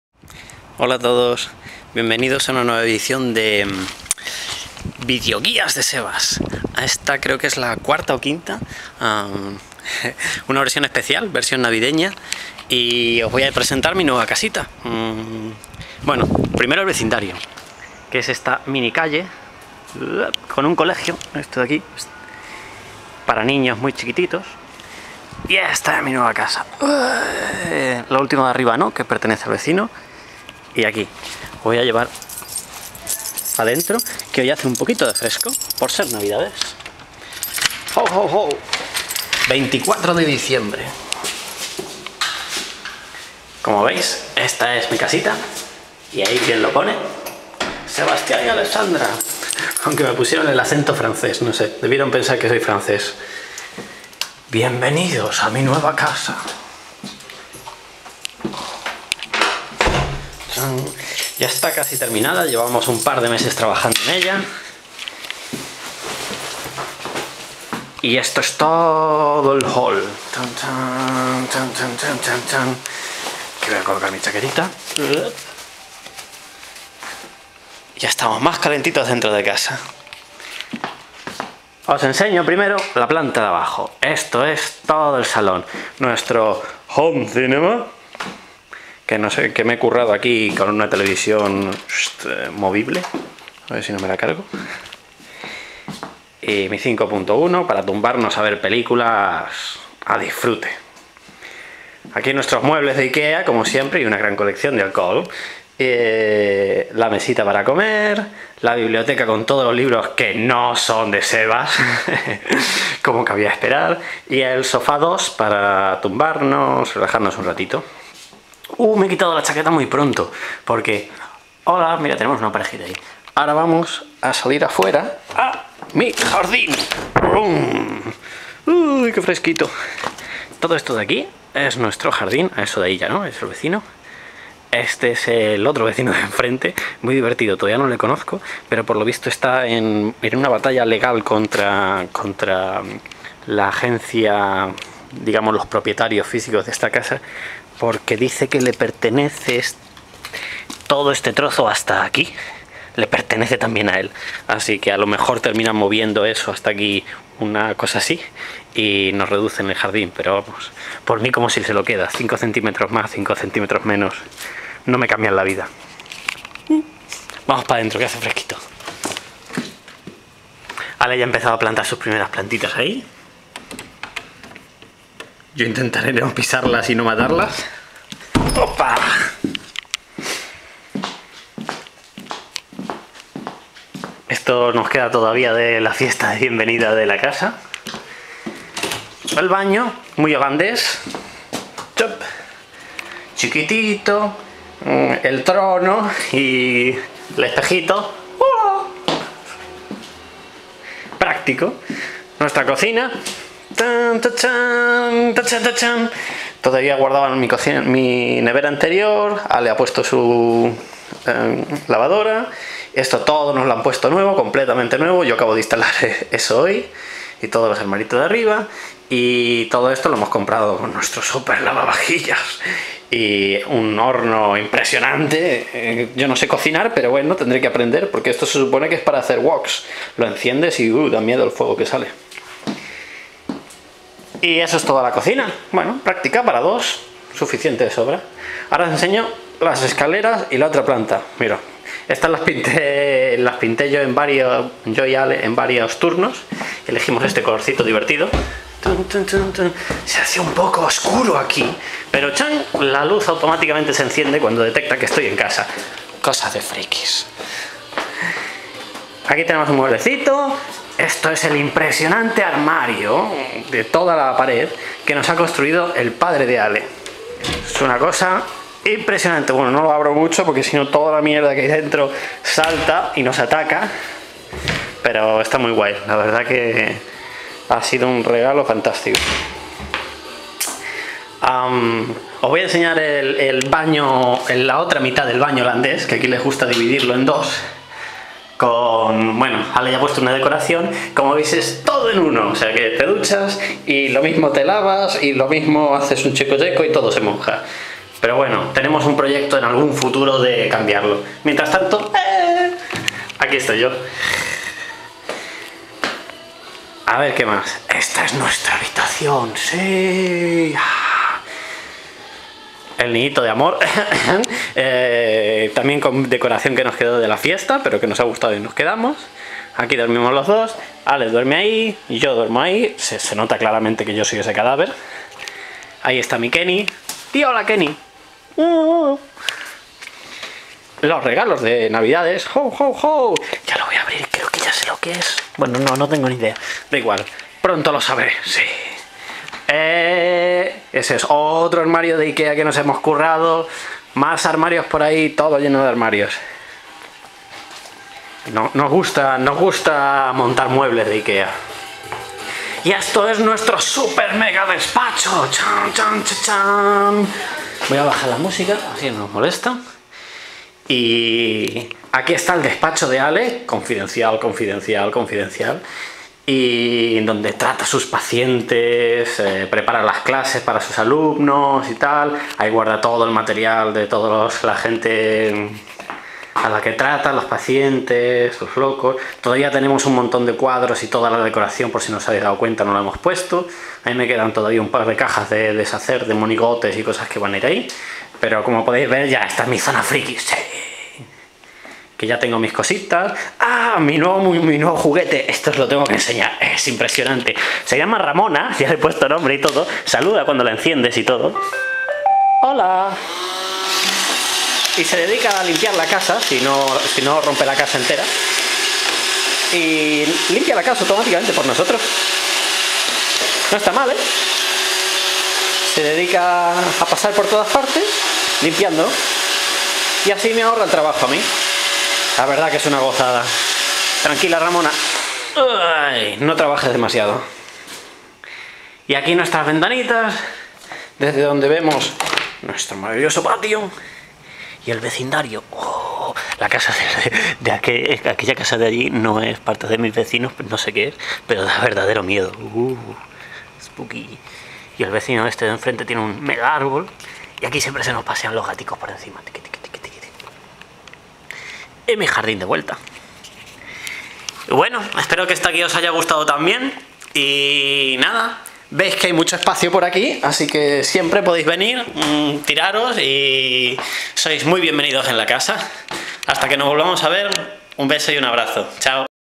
Video guia casa de Sebas y Ale
Video guía de la nueva casa de Sebas y Ale en Amsterdam